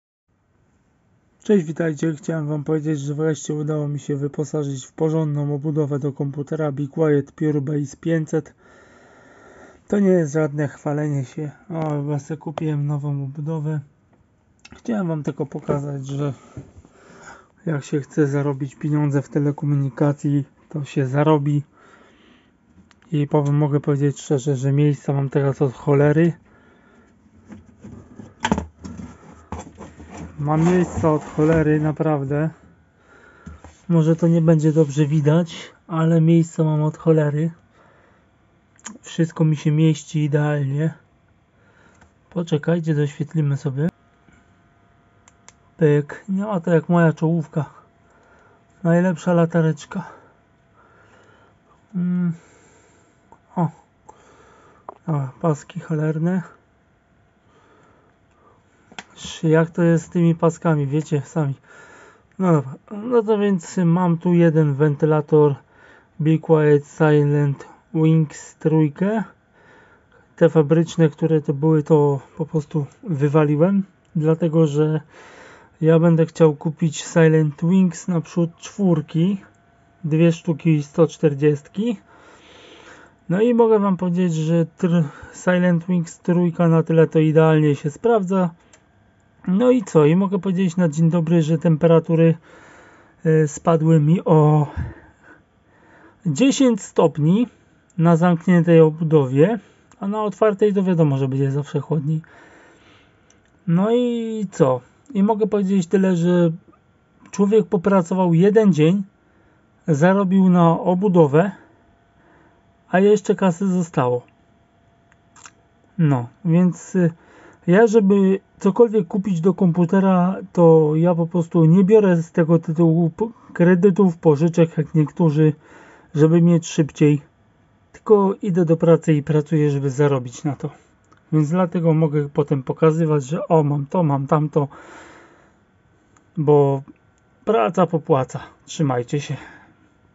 Obudowa komputerowa be quiet pure base 500
Obudowa komputerowa be quiet pure base 500 kupiona za około 400 zł . Warto było wydać taką kasę bo efekty są ogromne momentalne ogromny spadek temperatury przy zamkniętej obudowie ilość miejsca w obudowie gigantyczna możliwość zamontowania dodatkowych dysków też jest zwiększona.